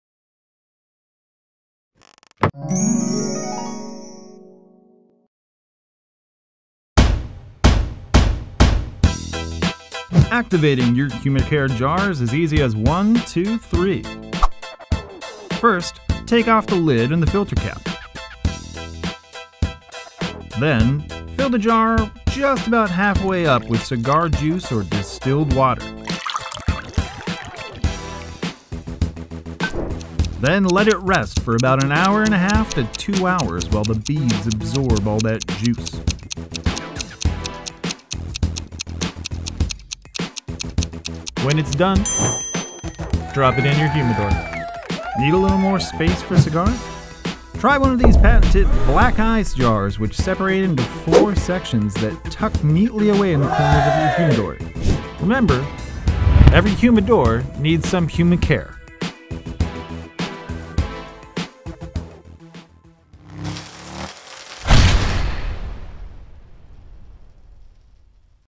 How to Activate Your HUMI-CARE Jar - Cigars International
Activating your HUMI-CARE Jar is as easy as 1,2,3 and will help keep your cigars fresh. Just follow these simple steps.